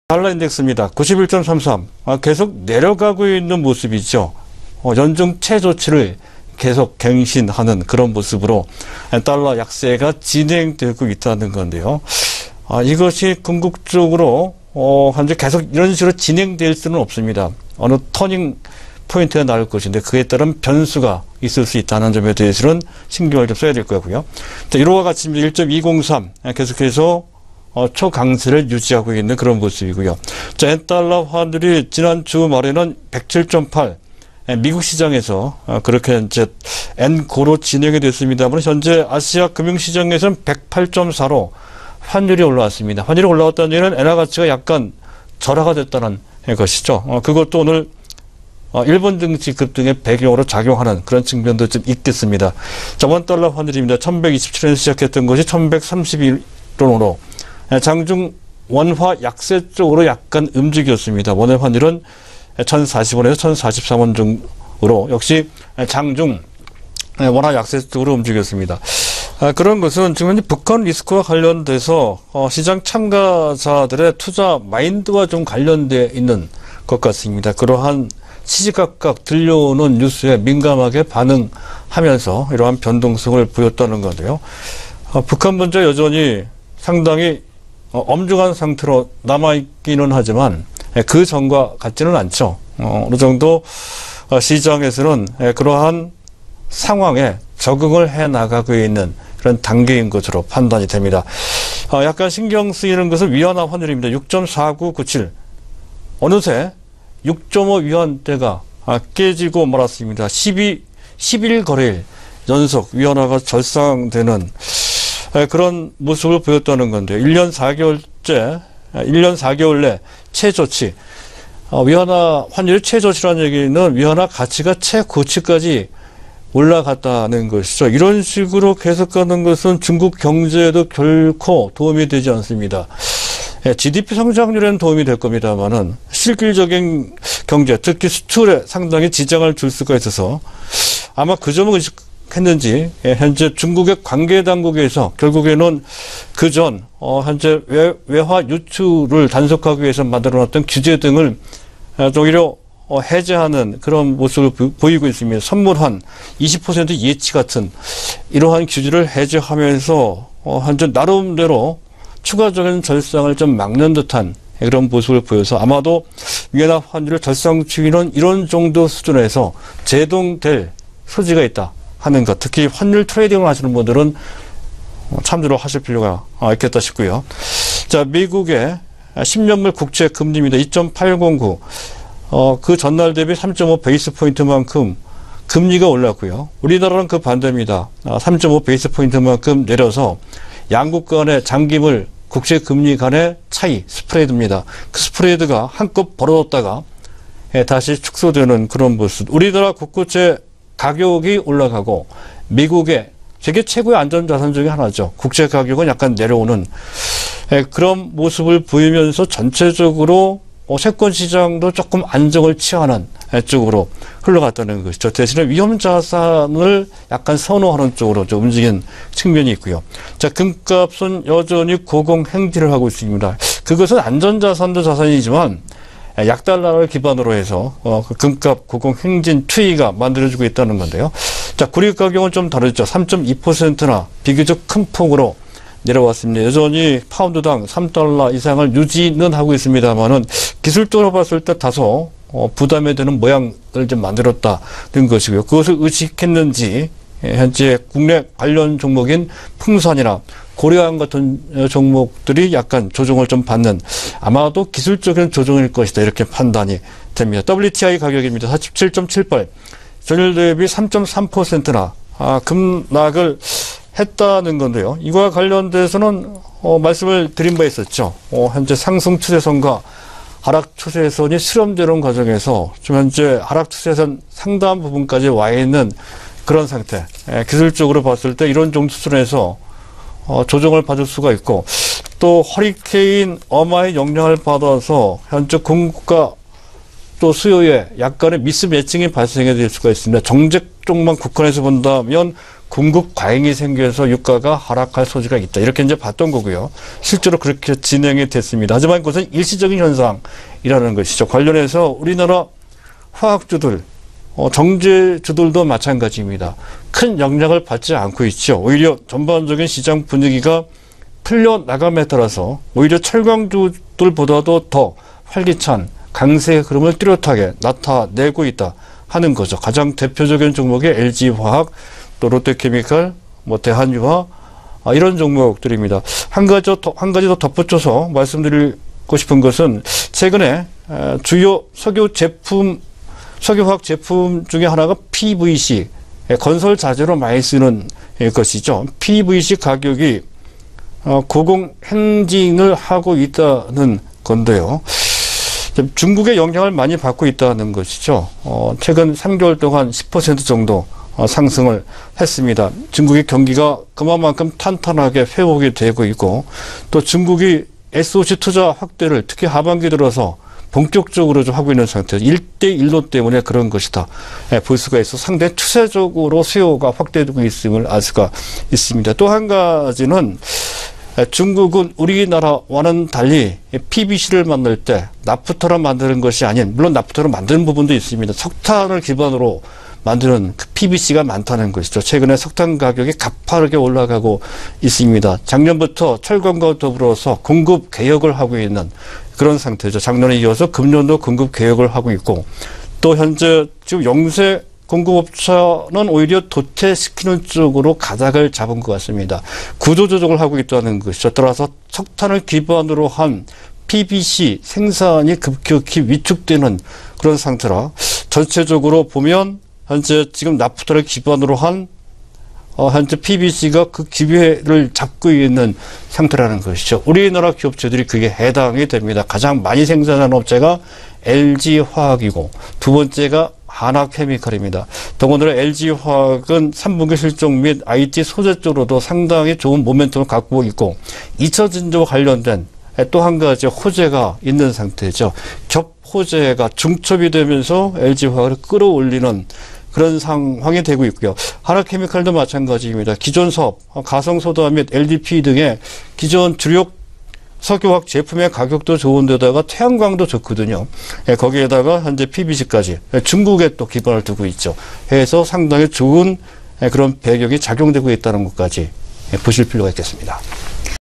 PVC 가격 고공행진, 수혜주 찾아보니?
환율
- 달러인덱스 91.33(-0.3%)
- EUR 1.203$(+0.1%), 엔달러 107.8엔(-0.6%)
- 원달러 1,127원, 원엔 1,040원(-10원)
- 위안화, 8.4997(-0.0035위안)··· 0.05%절상
- 11거래일연속 절상, 1년4개월 최저

국채
- 미국채 10년물 2.089%(+3.5bp)
- 국고채 10년물 2.240%(-3.5bp)
- 양국금리차 축소 → 위험자산 up

유가·금
- 금 +0.1%, 약달러 기반
- 구리 -3.2%

WTI 47.78$(-3.3%)
- 허리케인 "어마" 로 정제시설 가동중단
- 일시적 "미스매칭"에 의한 공급과잉

PVC 가격 고공행진
- 3개월만에 10%상승, 톤당 955$
- 중국 모멘텀
- 경기회복, SOC투자(일대일로)
- 석탄기반 PVC 단가상승

수혜주
- LG화학 : 3분기 실적 + IT소재 + 2차전지
- 한화케미칼 : CA, LDPE + 태양광